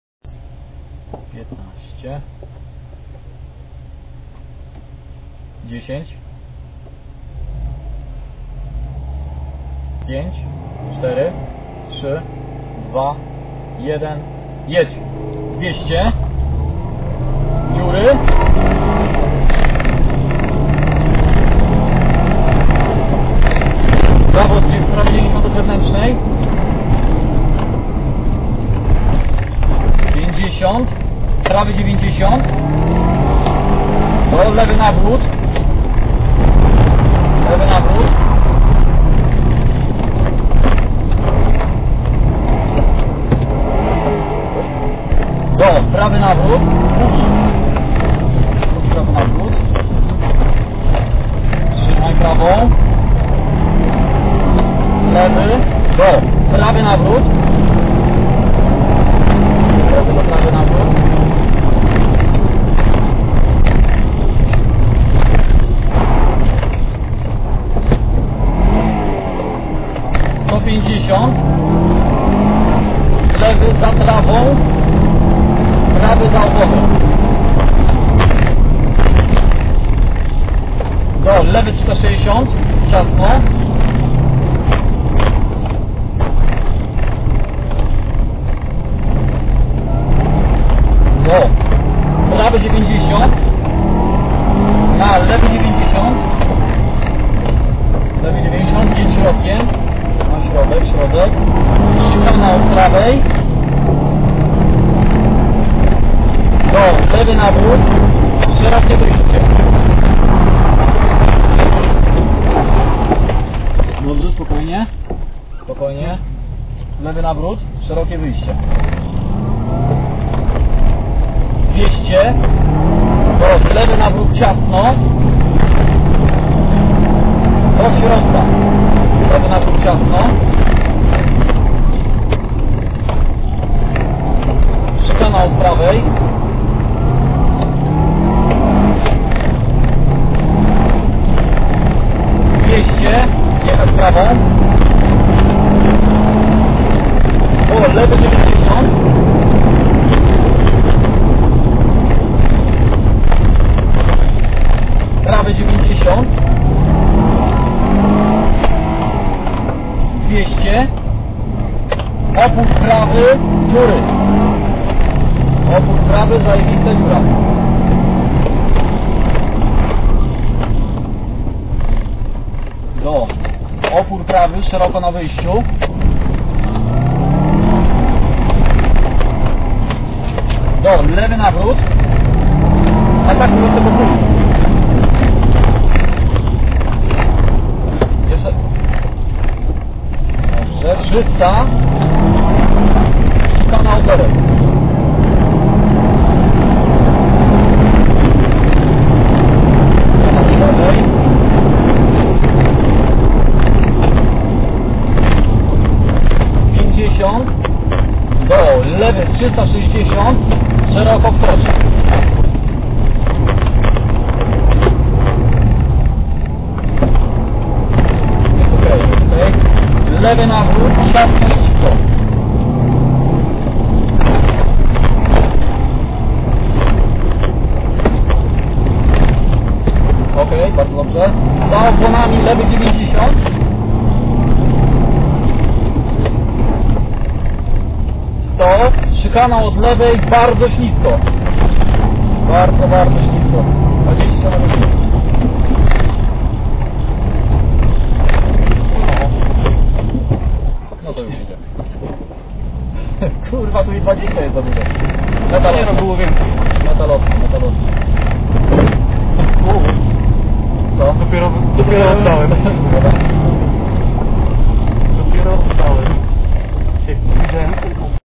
Milano Rally Sprint - Orneta - 2012.05.06 - festiwal ;-)
Festiwal błędów.

2x jedynka (a w zasadzie luz), bezsensowny spin w dziwnym miejscu i szykana na koniec...
Uczcie się jak nie należy jeździć. ;-P

Czwarty (ostatni) przejazd "na ostro".